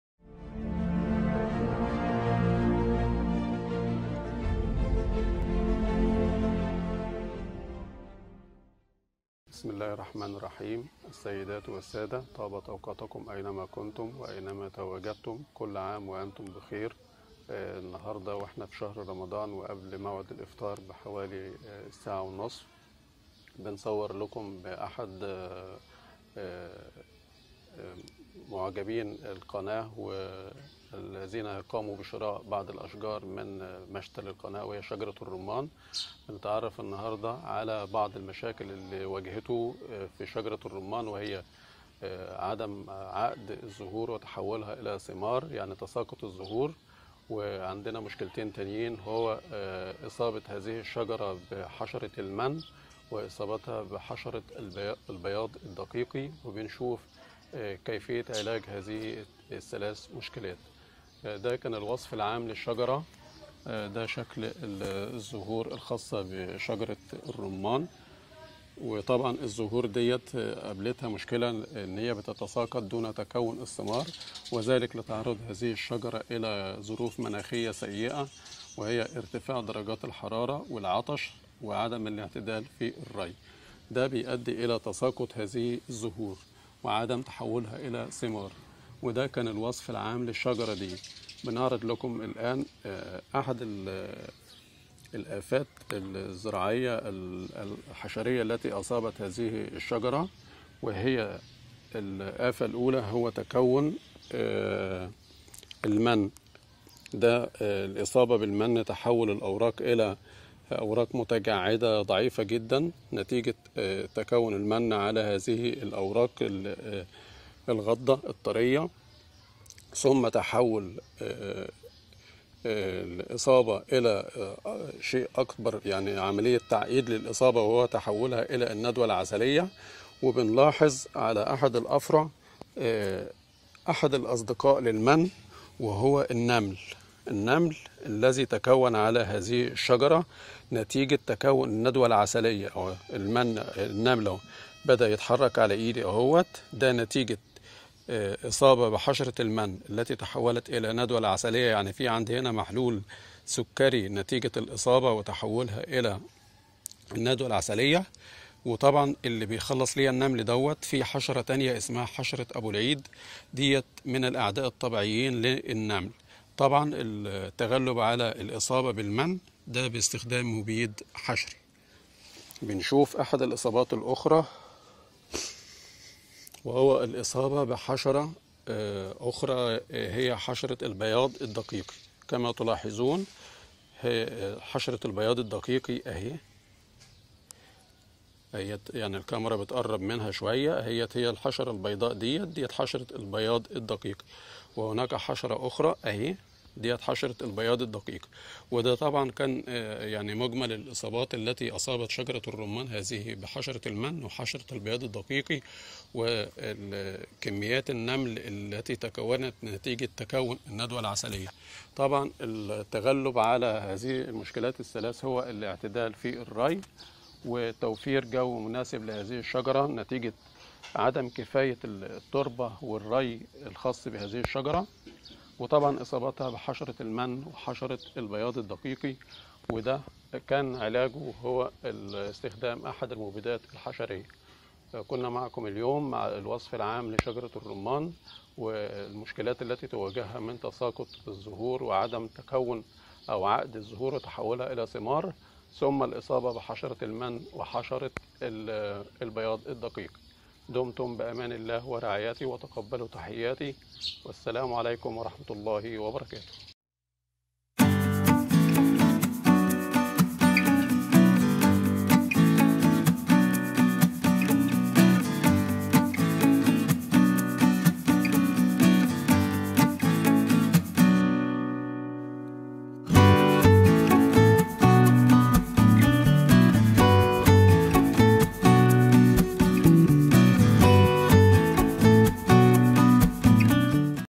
نبات الرمان الوصف العام / المشاكل و الآفات التى تصيبها و طرق الحل و الوقاية منها
◄السلام عليكم اهلا ومرحبا بكم في قناة الزراعة المنزليه فن وجمال للدكتور اشرف
الصياد .

◄نعرض لكم اليوم : نبات الرمان الوصف العام/ المشاكل و الآفات التى تصيبها و طرق الحل و الوقاية منها